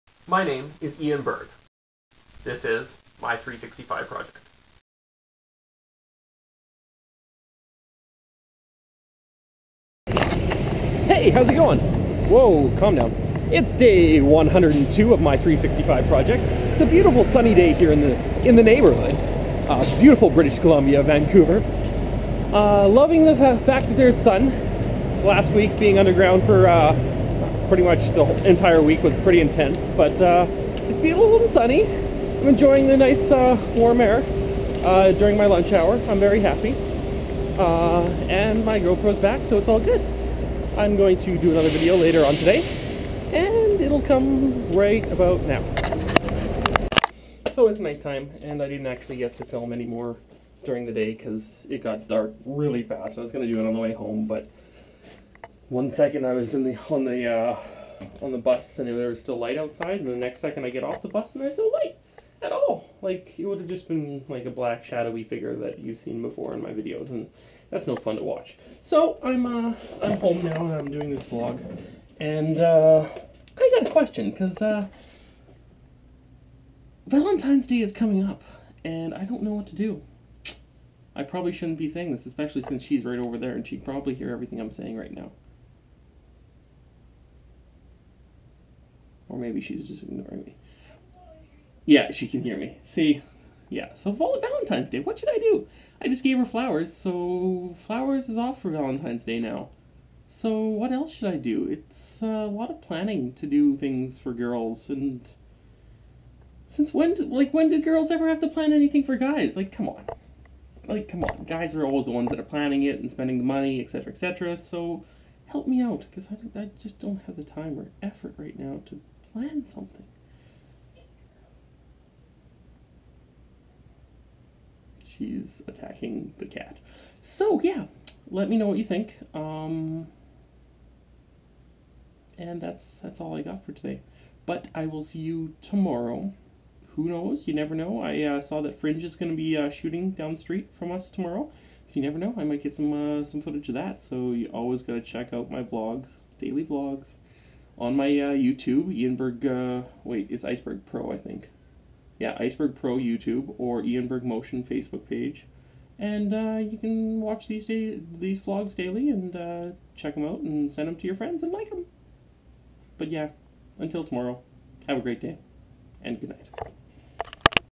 Day 102 of my 365project - sunny day!
a sunny day in the neighborhood!? and questions about Valentines Day.